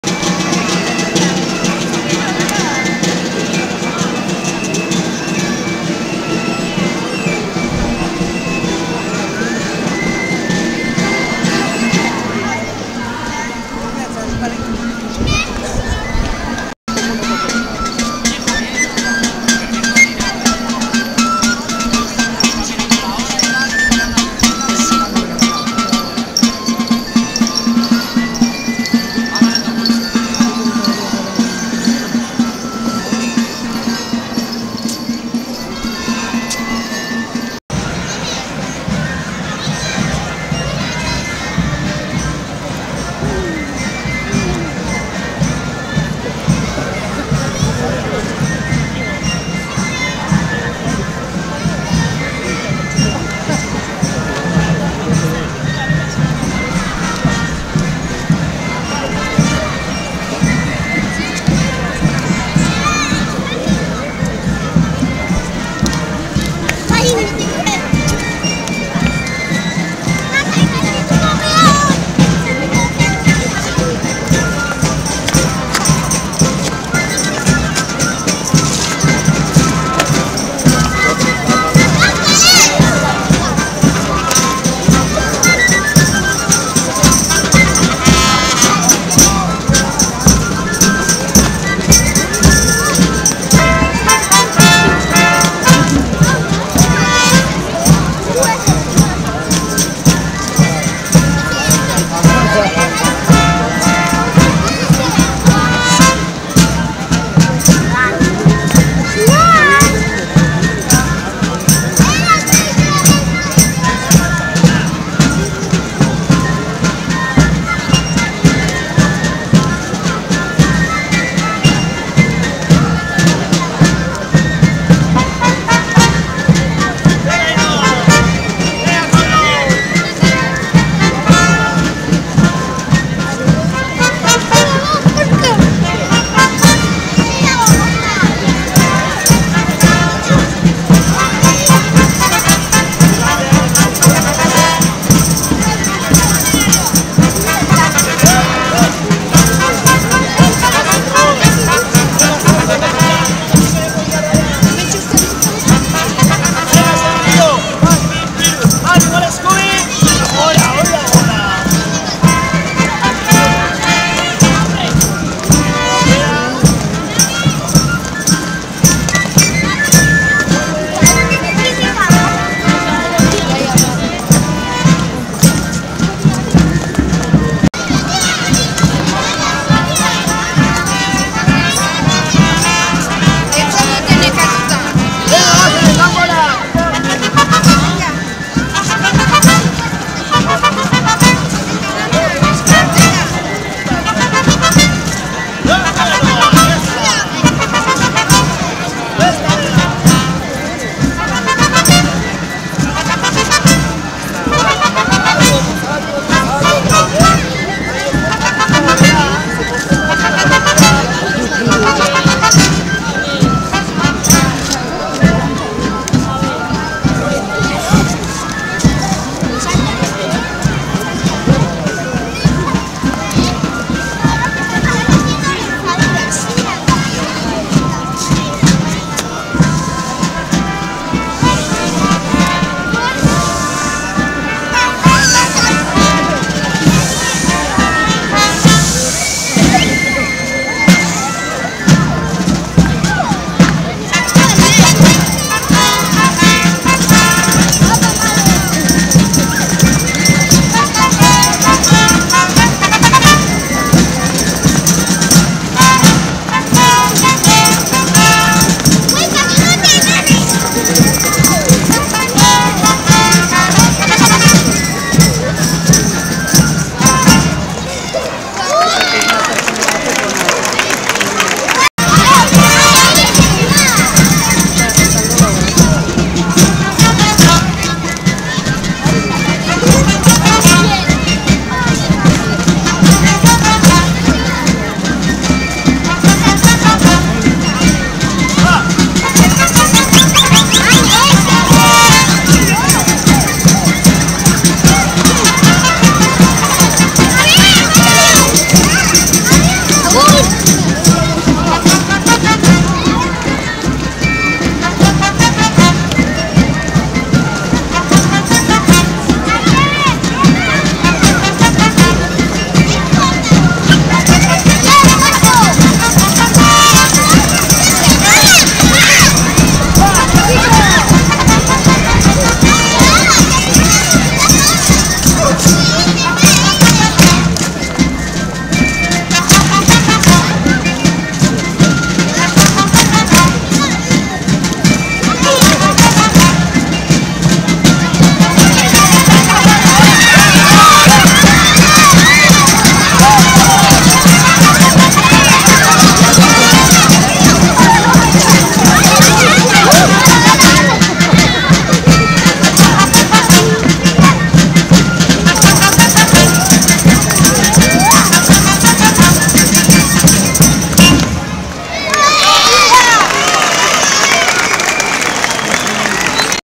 Inauteriak Donostia 2014-2-27
Inauteriak Donostian Eskola, Kresala eta Trebeska dantza taldeak plaza Berrian